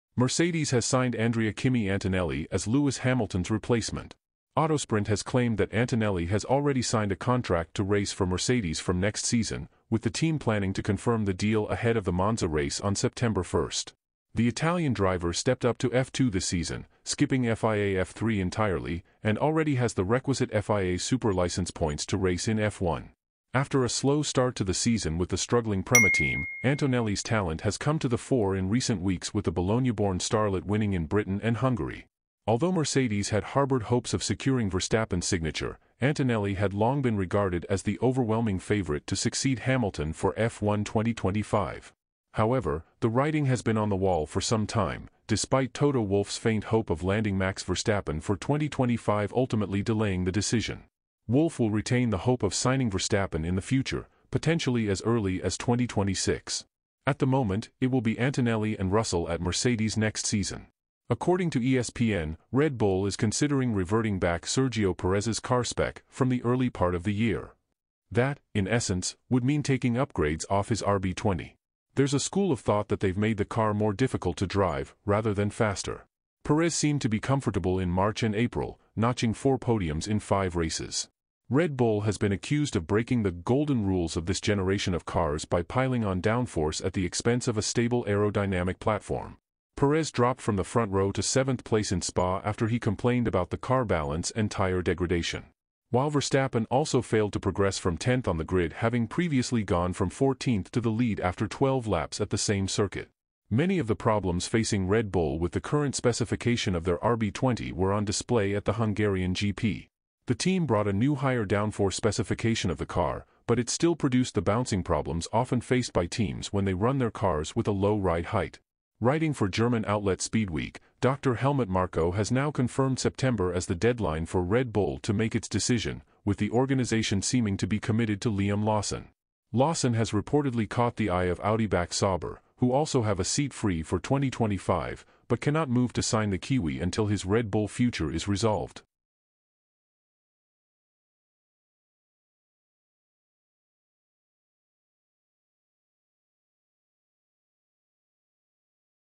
KIMI ANTONELLI REPLACES LEWIS HAMILTON FOR NEXT SEASON
ANTONEELI AND RUSSELL TO LEAD MERCEDES NEXT SEASON. PEREZ SOLUTION